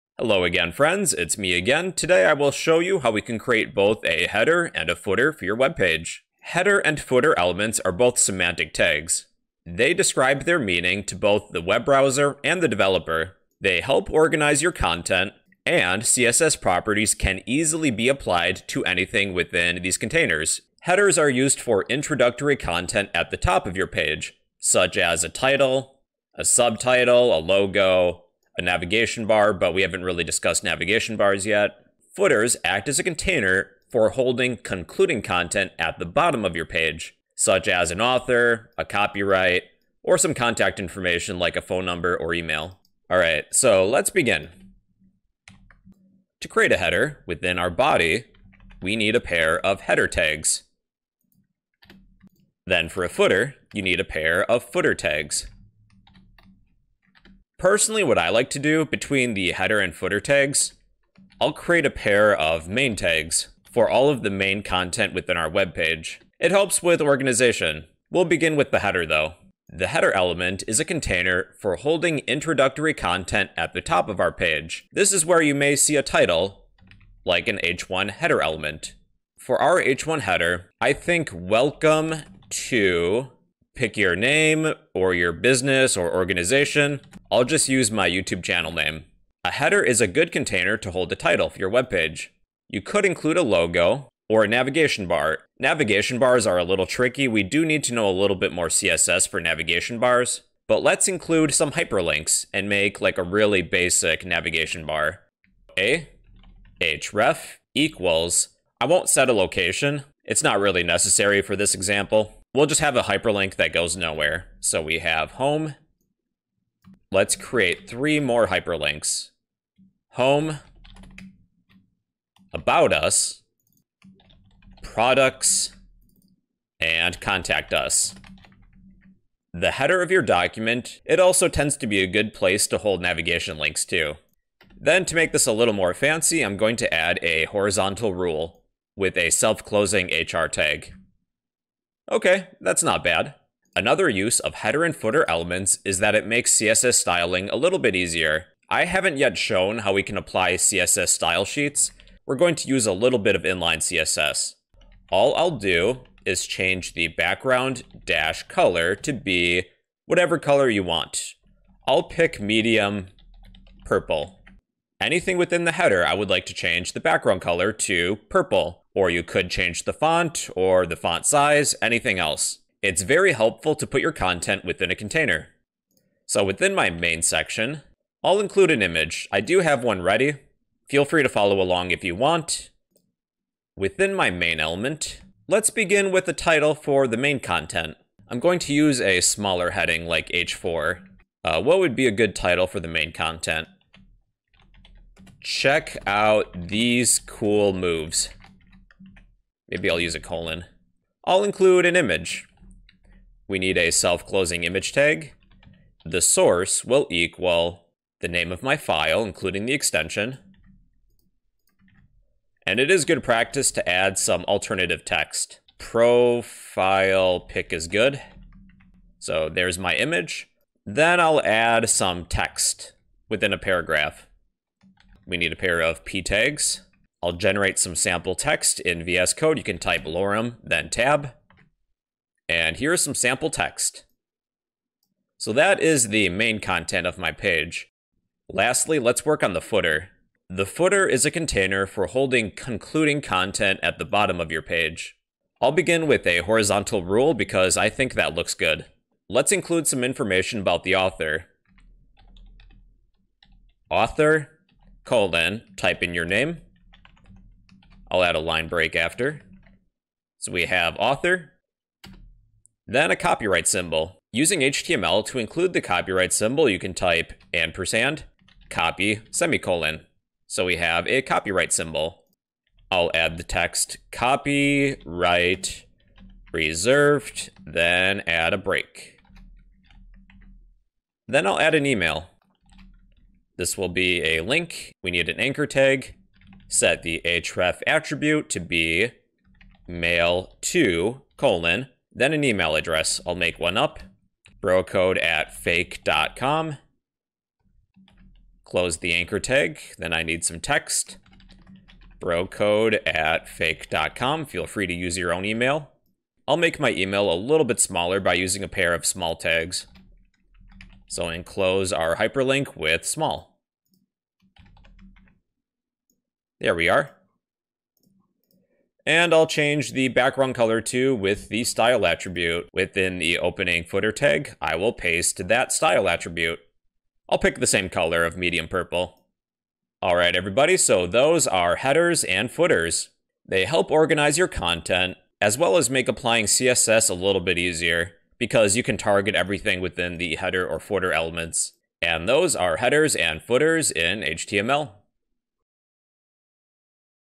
Learn HTML headers & footers in 5 minutes! 🤯
HTML header footer tutorial example explained

00:00:00 introduction
00:01:09 header
00:02:55 main
00:03:57 footer
00:05:29 conclusion